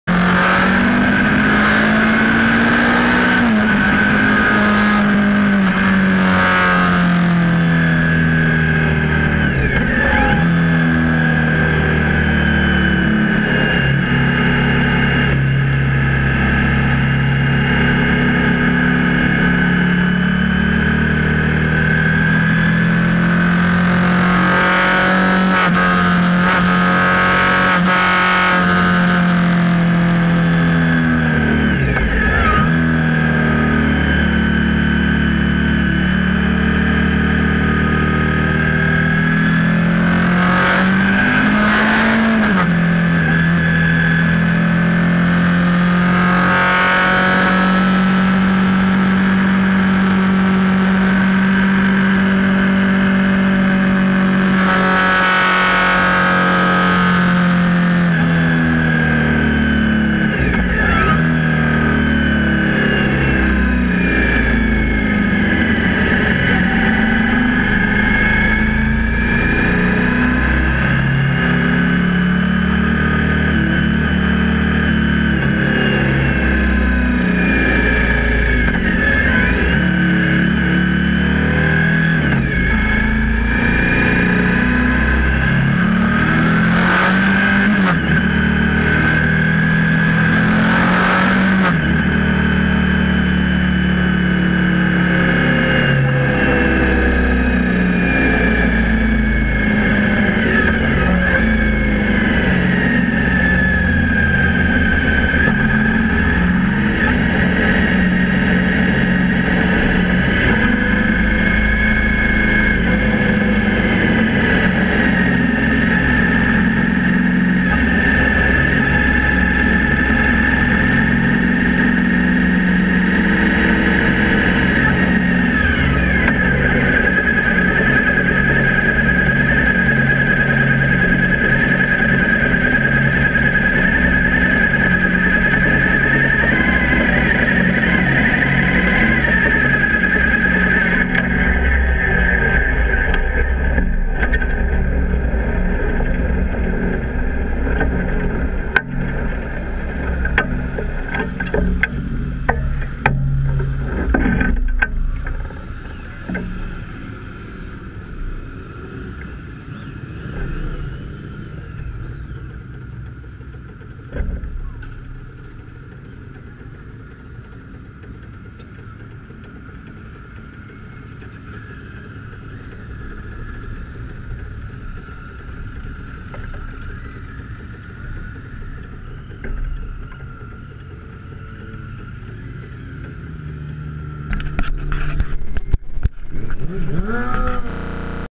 The Ridge Motorsports Park-7/4/16-Onboard with Randy
Video 4-The Ridge Motorsports Park-7/4/16-Onboard with Randy
Video 4. Randy on his V-Strom 1000. OPRT level 1.